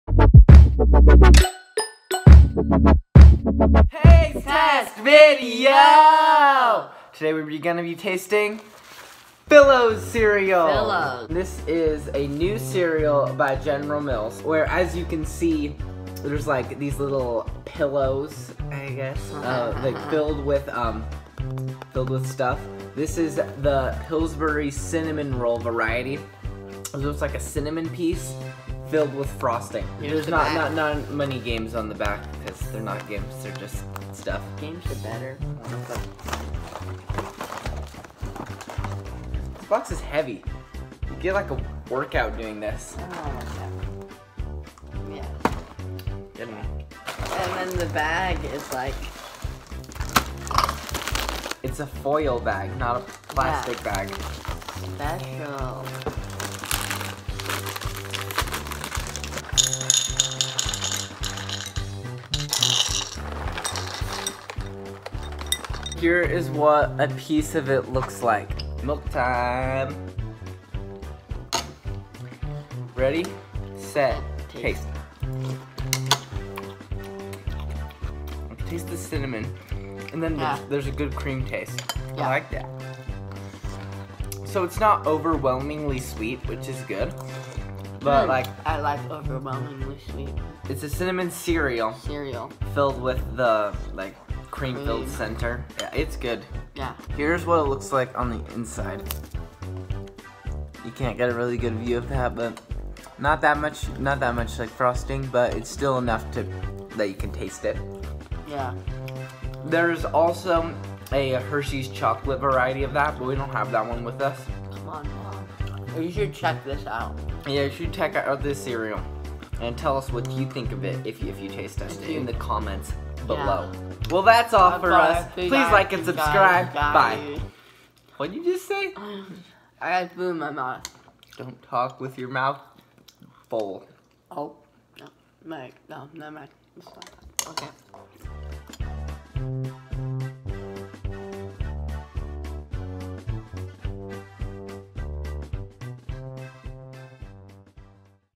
Pilsbury Cinnamon Roll Fillows Cereal Taste Test
In this video we will be tasting Fillows Cereal by General Mills.